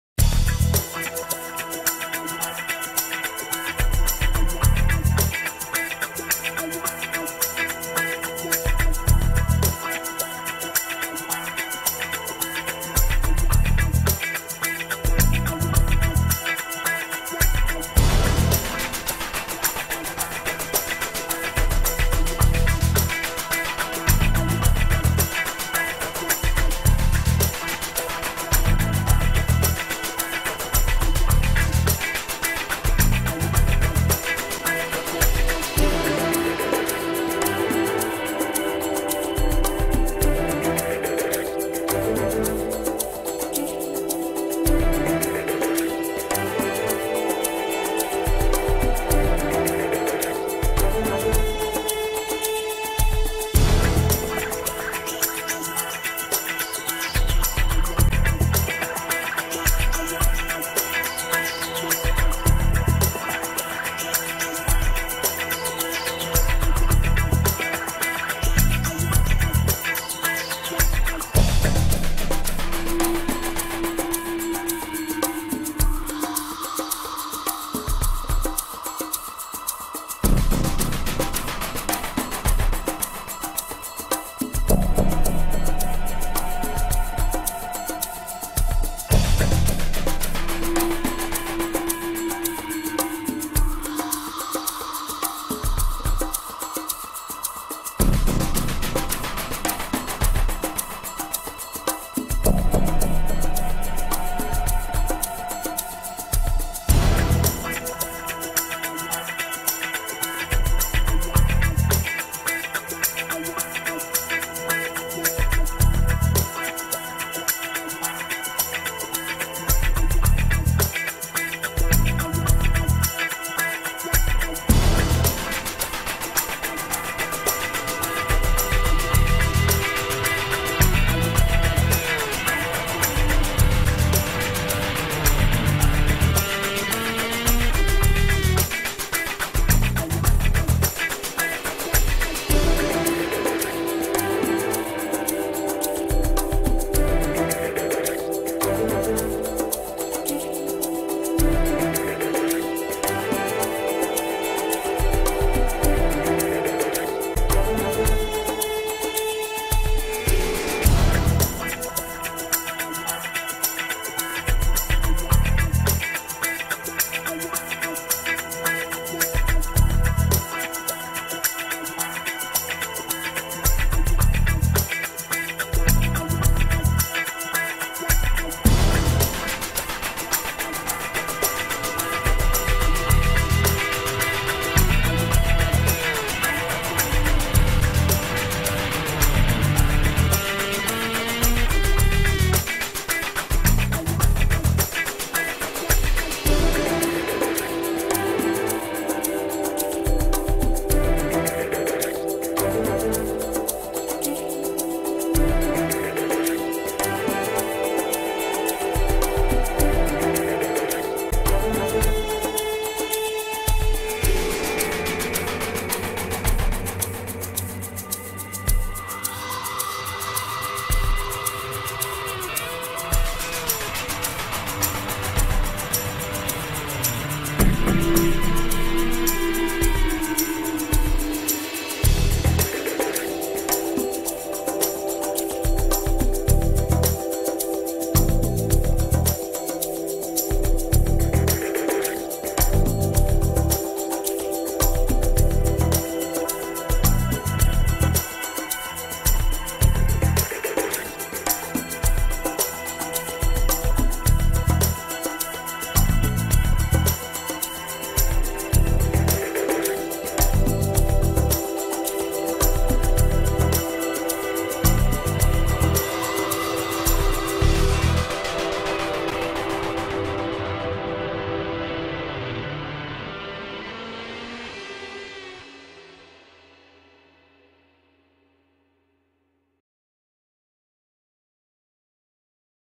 AquaSource 25L Water Tanker
Présentation de l'Aquasource 25 L destinné à remplir des réservoir Source.